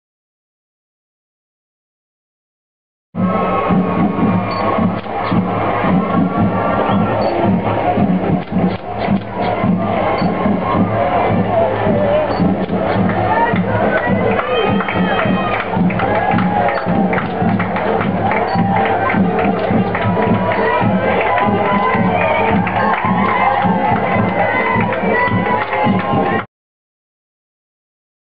Olympic Torch Passes Sound Affects Music 2012 - Sound Affects Archive
Great day at Sound Affects Music 01/05/2012 as the Olympic Torch passes right past our window.

Sound Affects Music
Ormskirk